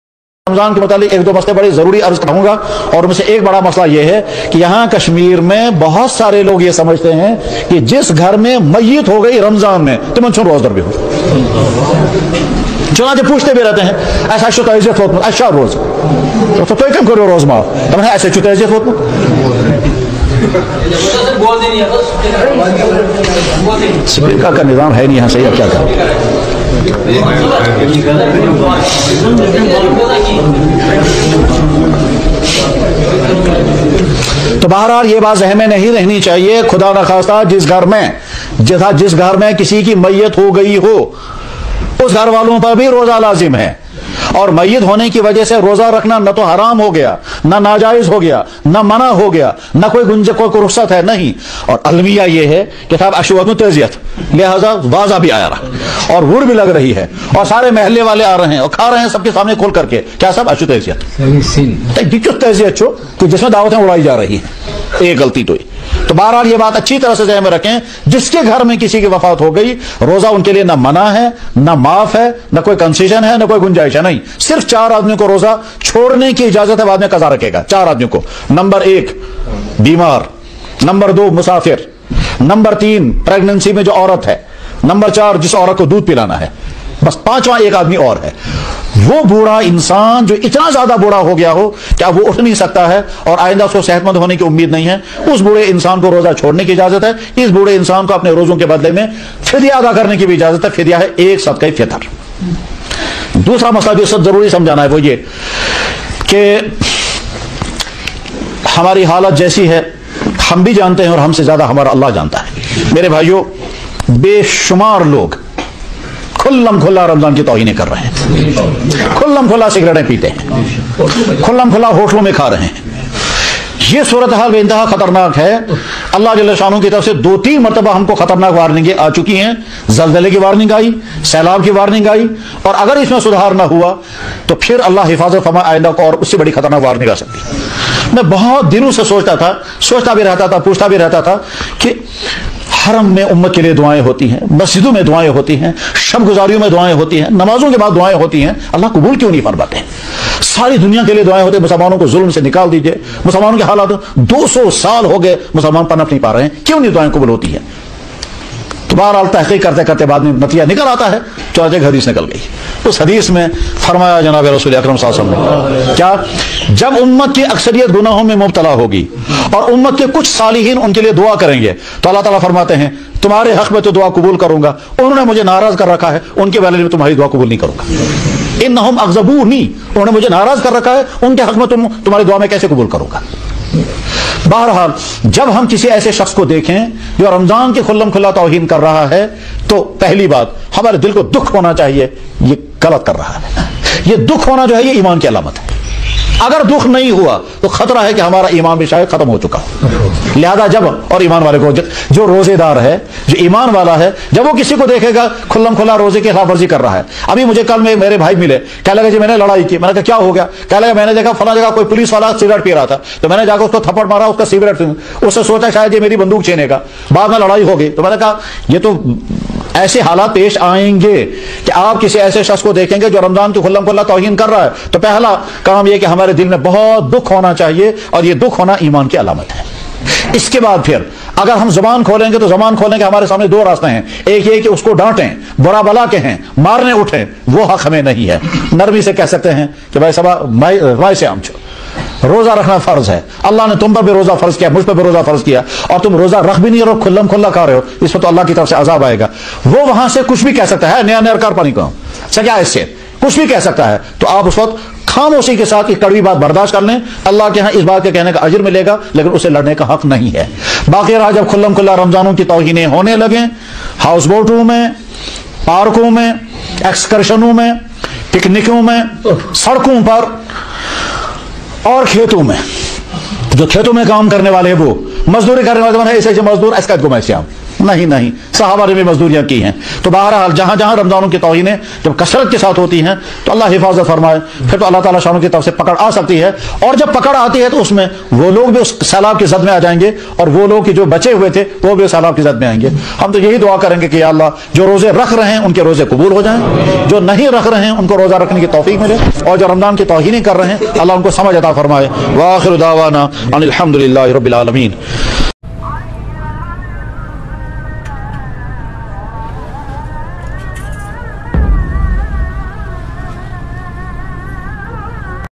Sc-12 Masail_e_Ramzan(رمضان کے مسائل)Mufti Nazir Ahmad Qasmi-रमजान की समस्याएं-রমজানের সমস্যা
▂ ▃ ▄ ▅ ▆ ▇ █ 【Tawwabeen_Production】 █ ▇ ▆ ▅ ▄ ▃ ▂
Masail_e_Ramadhan
If someone dies in Ramadan,is fasting not compulsory for bereaved family |Concession in Fasting for 5 persons || Hazrat Mufti Nazir Ahmad Qasmi Sb Db
مشاكل رمضان